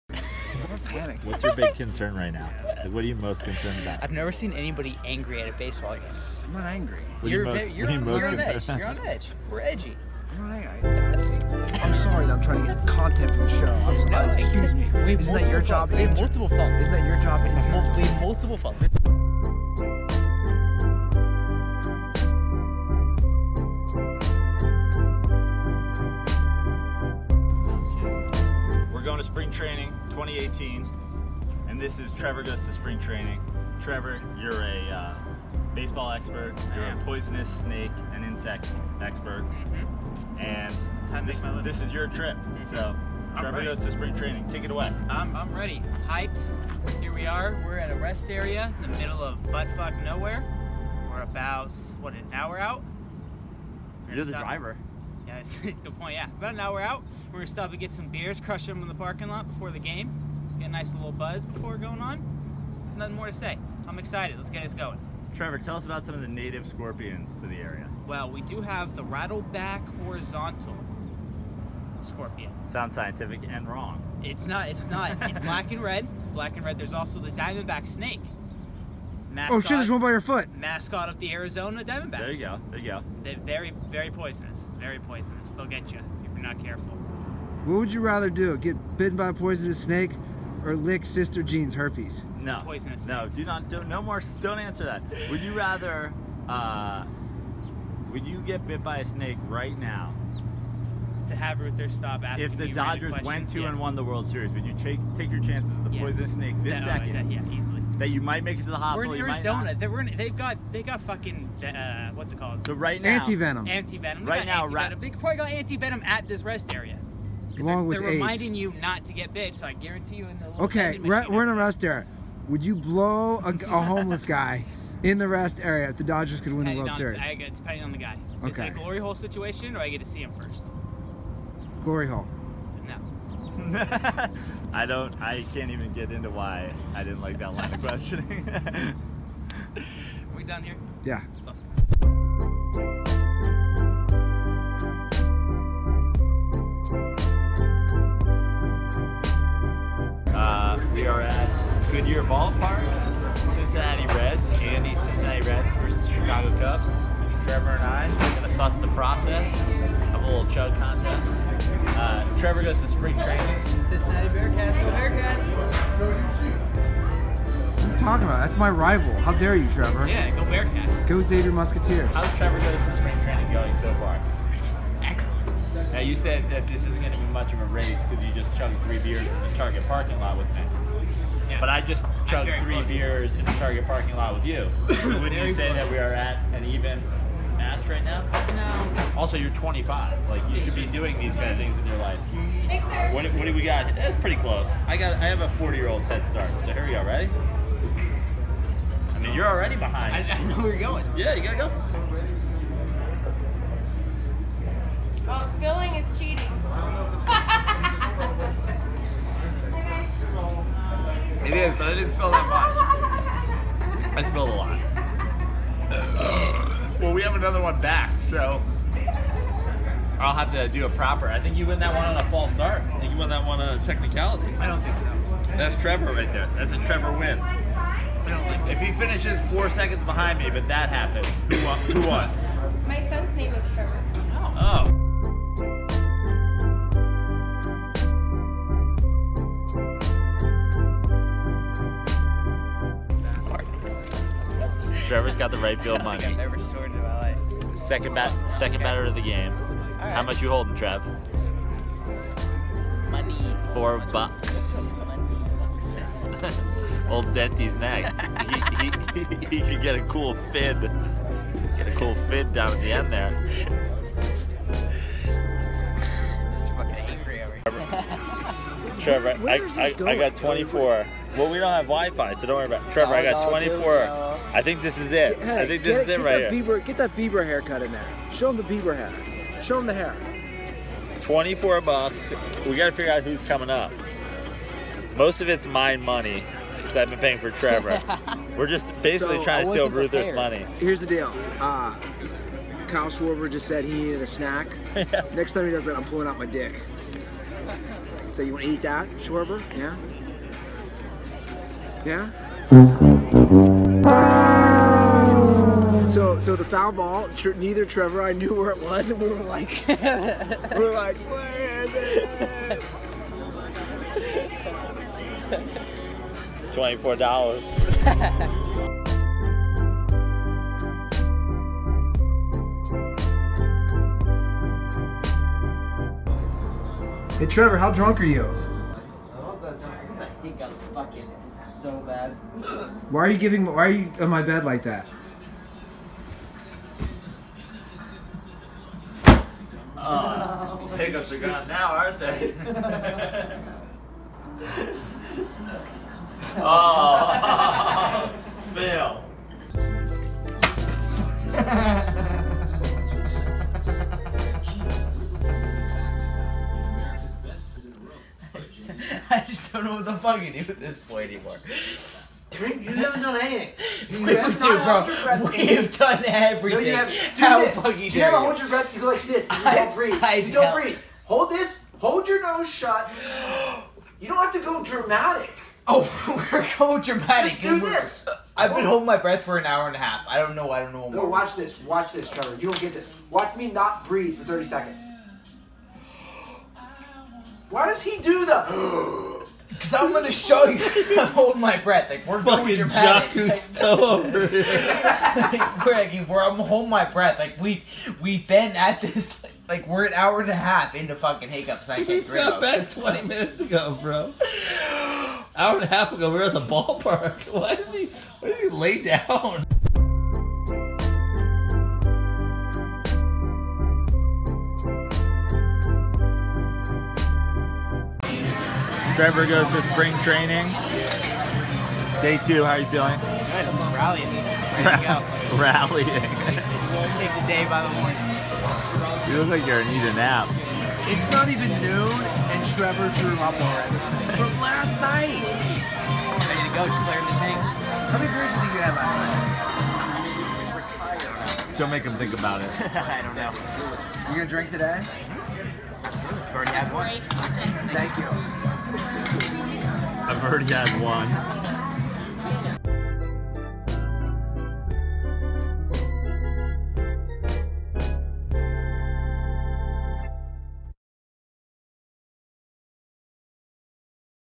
DS Roadtrips: Intern Trevor Goes to Spring Training
Intern Trevor goes to Spring Training in Arizona